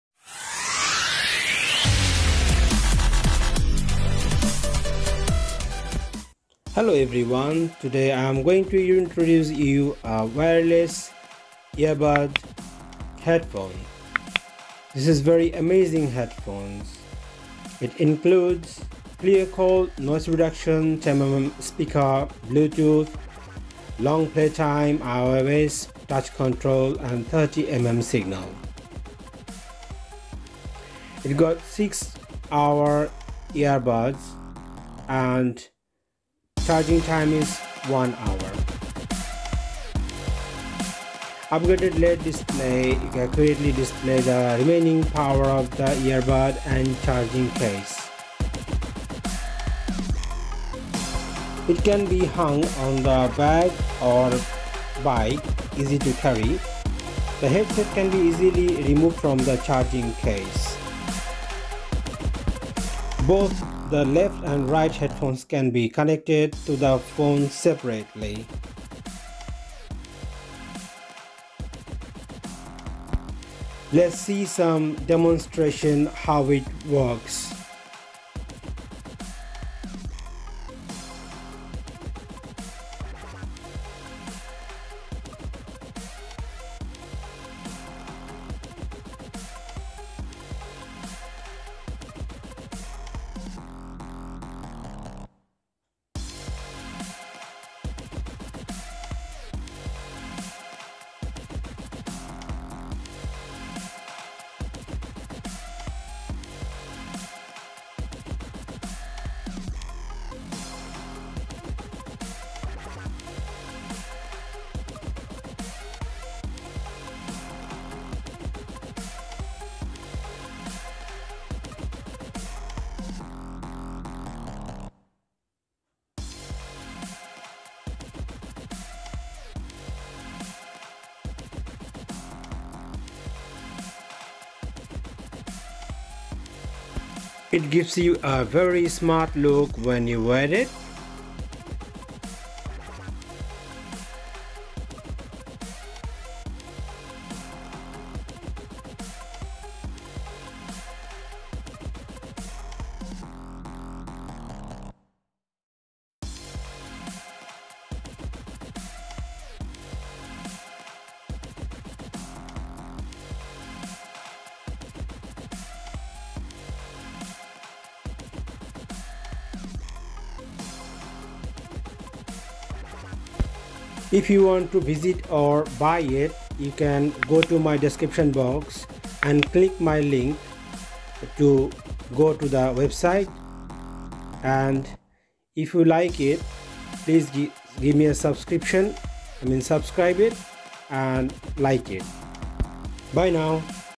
Wireless earbud headphones || Tiksounds || uk
Amazon associates

Wireless Earbuds Headphones with Charging Case,
Immersive Bass Sound, Single/Twin Mode, Built-in Mic,
150 Hrs Playtime, IPX7 Waterproof, for Work/Sports/Running

Brand: Tiksounds
Connections:Wireless
Colour: Black
Headphones Form FactorIn EarItem
Weight: 91.5 Grams
Water Resistance Level: Waterproof